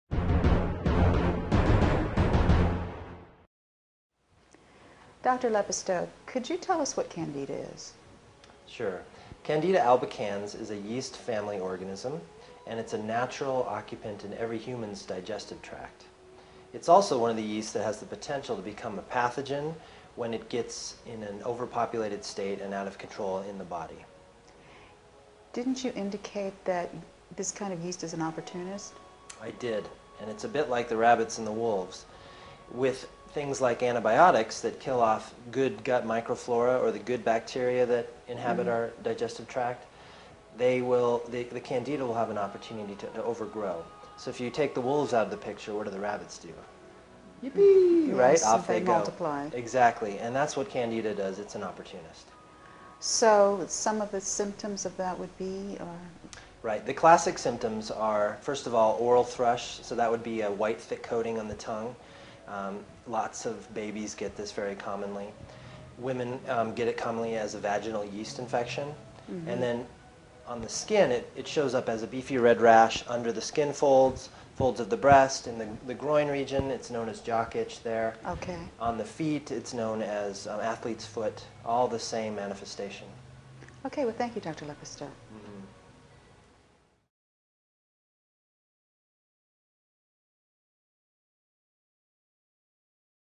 Candida
Dr. Christopher Lepisto shares what Candida is and lists the illnesses that it can cause.